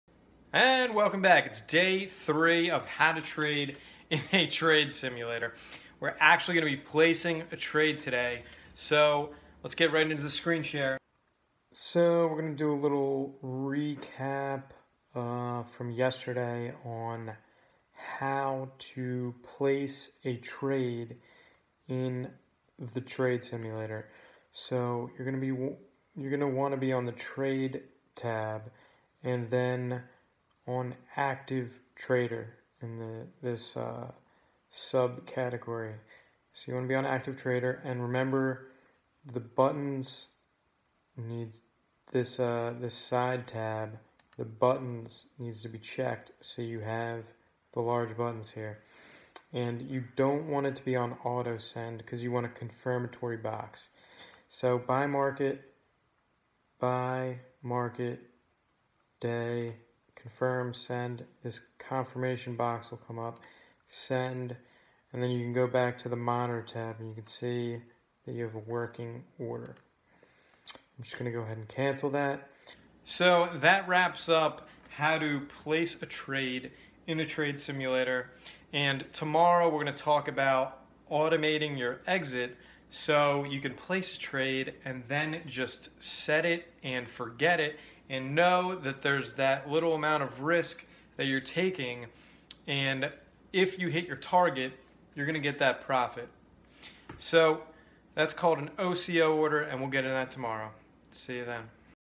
How to place a trade in a trade simulator - Thinkorswim TD Ameritrade software - Day 3 (of 5)
Inflection Point Inc. founder created this five day crash course on how to trade in a market simulator with a goal to provide you with new techniques and approaches for trading in your market, while keeping them as actionable and succinct as possible. Day 3 we go over placing a trade in the simulator thinkorswim from TD Ameritrade.